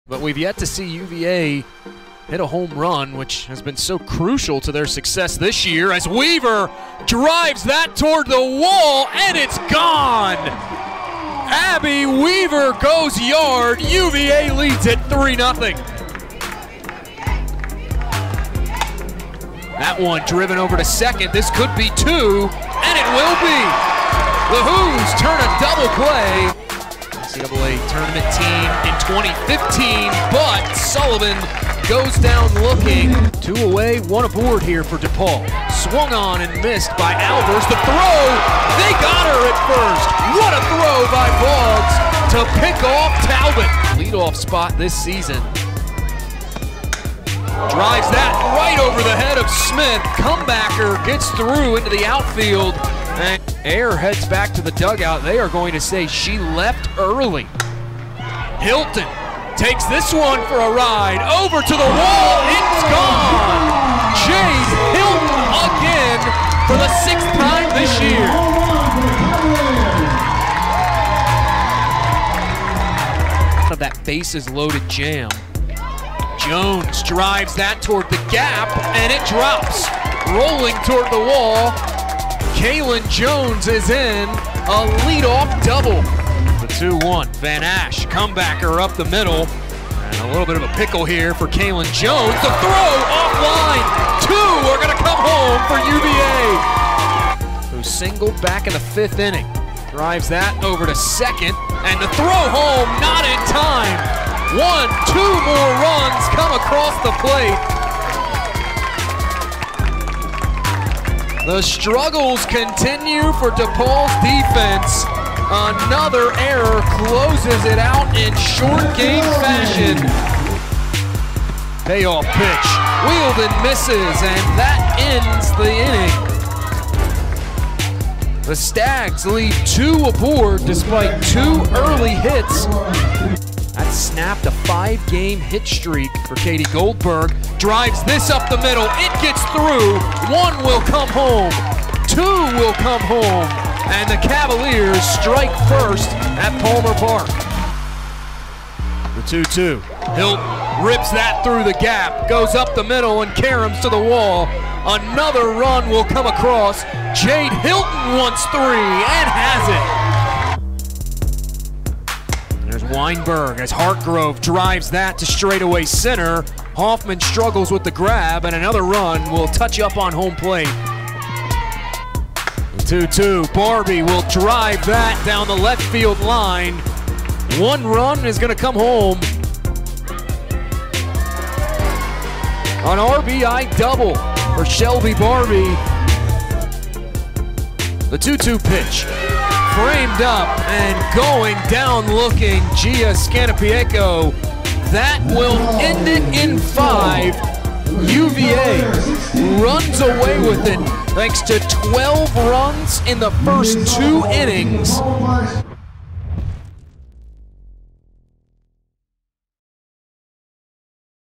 SOFTBALL - DePaul / Fairfield Highlights
The Cavaliers sweep through the first day of the Palmer Park Clash.